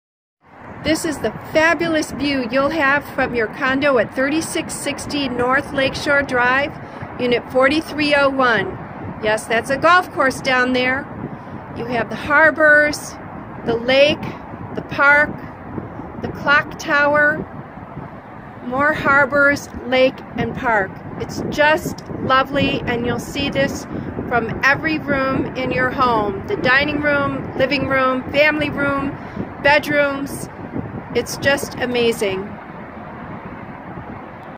Your Amazing View at 3660 N. Lake Shore Drive
See the Skyline, Belmont Harbor, parks, Lake, Montrose Harbors, Montrose Golf Course and more. Beautiful view at night, too.